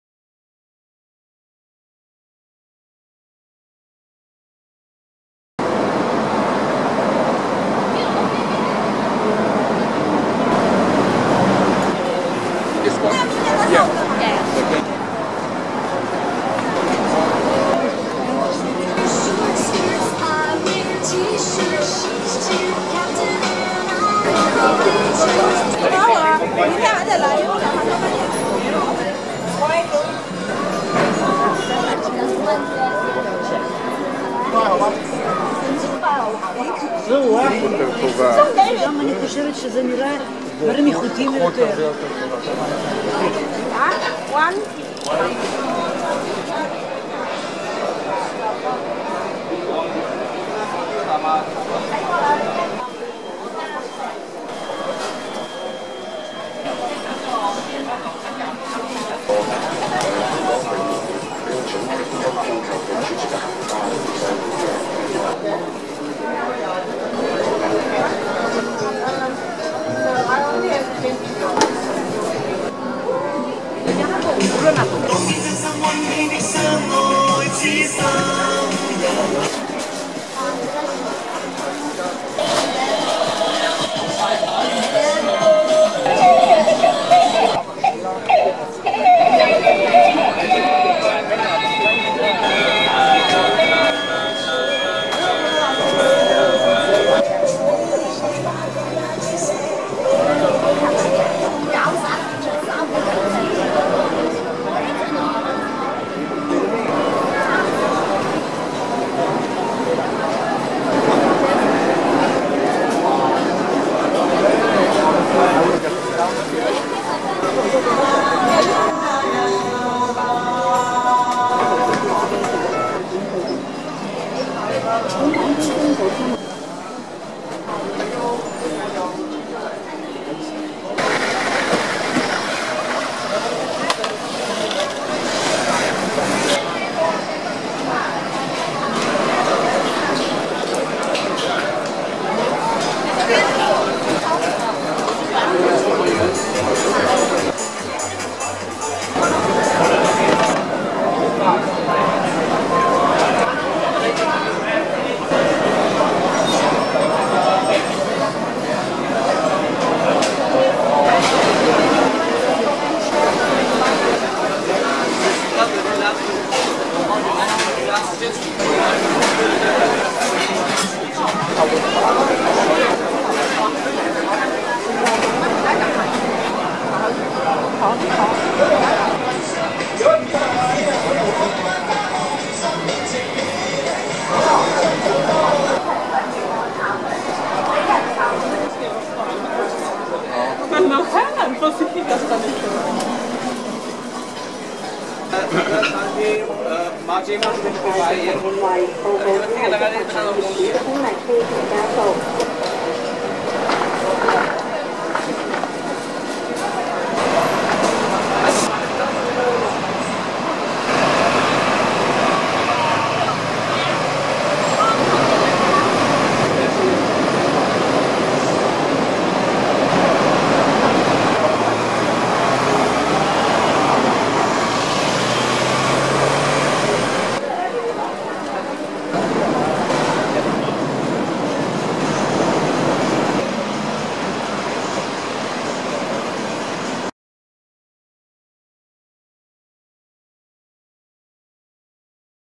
Temple Street Night Market Hong Kong (March 2016)
The Temple Street Night Market is located in the heart of Yau Ma Tei in Kowloon, Hong Kong;
Temple Street Night Market offers , clothing, souvenirs, jade, antiques, electronics, watches and a local cuisine;
(March 2016)